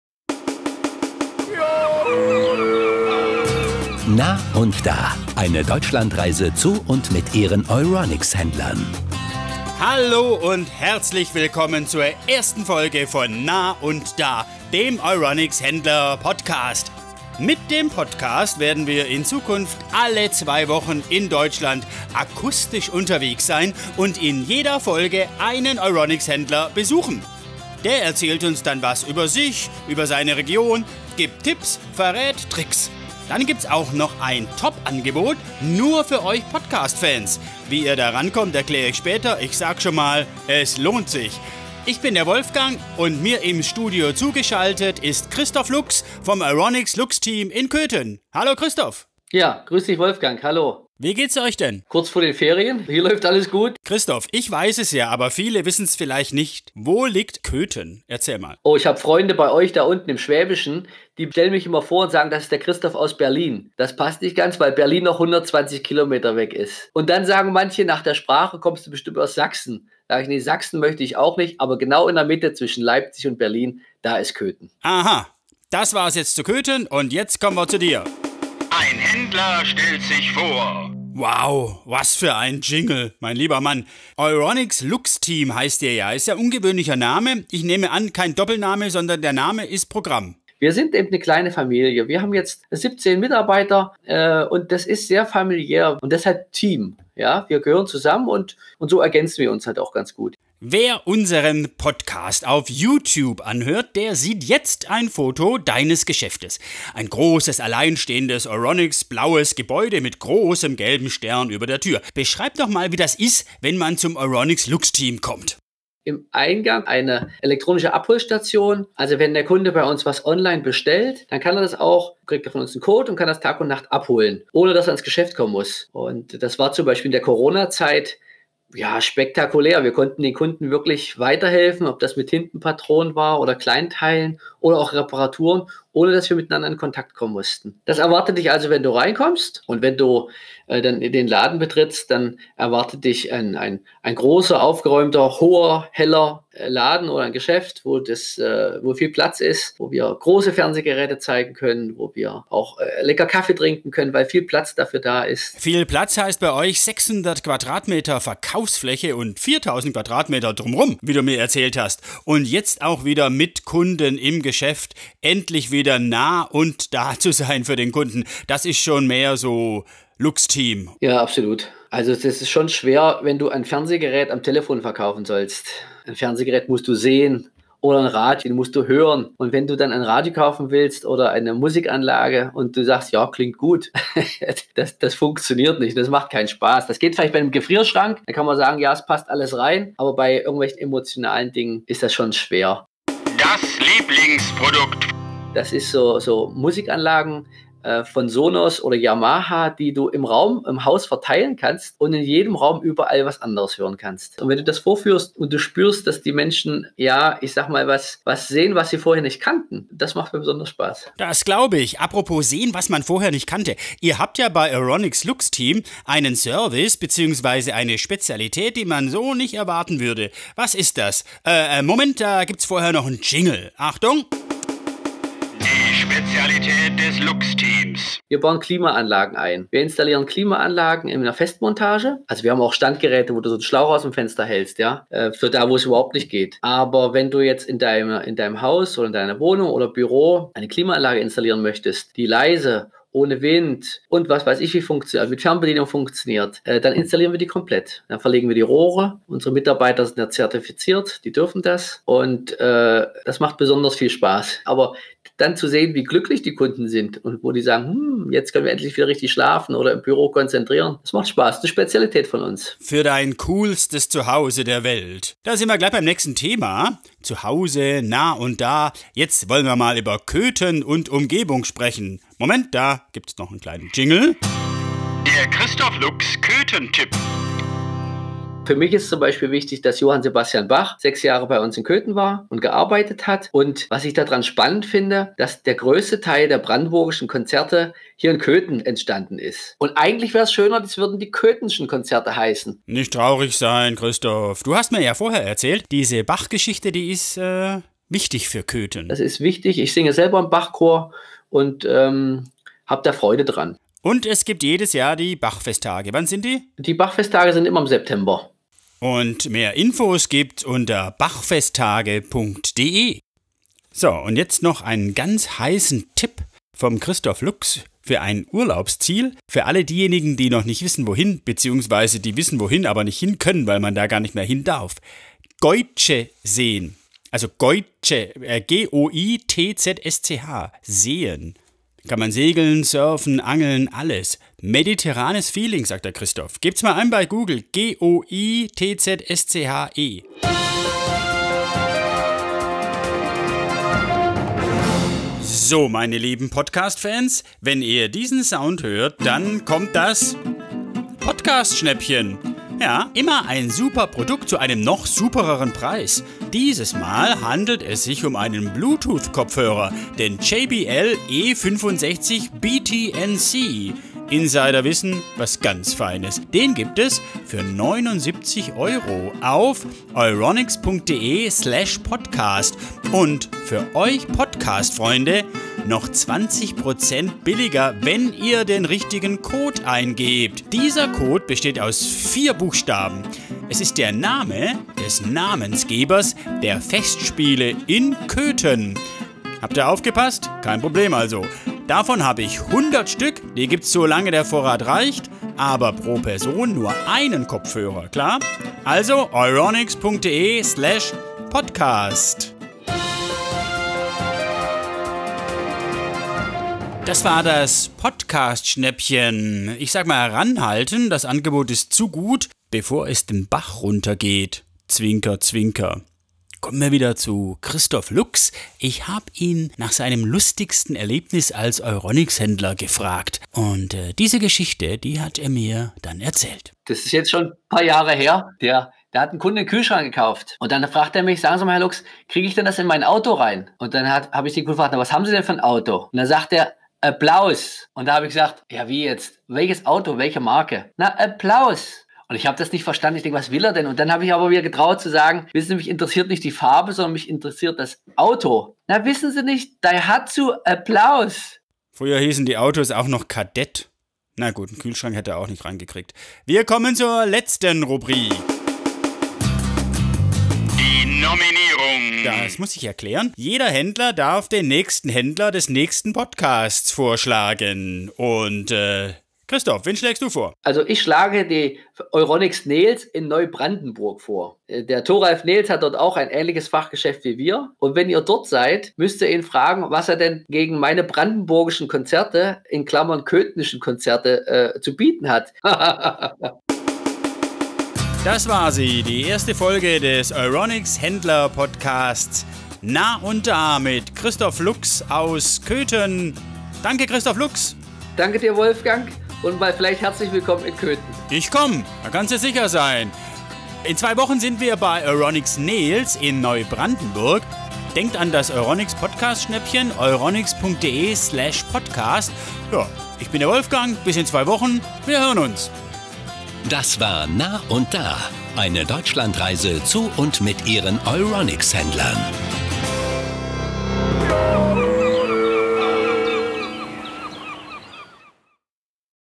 #1 - EURONICS Lux-Team über Klimaanlagen und 24h-Abholservice - EURONICS Händler Podcast
EURONICS Lux-Team ist in Köthen eine Institution. Und das nicht erst seit man sich einen schmucken Neubau gegönnt hat. Christoph Lux ist ein Köthener durch und durch, der seine Stadt liebt und kennt wie kein Zweiter. In seinem Podcast erzählt er uns, warum EURONICS Lux-Team EURONICS-Lux-Team heißt, was im September Köthen besonders macht, dass es in der Nähe von Köthen ganz mediterran sein kann und dass es die Musikgeschichte in Sachen Köthen nicht genau nimmt.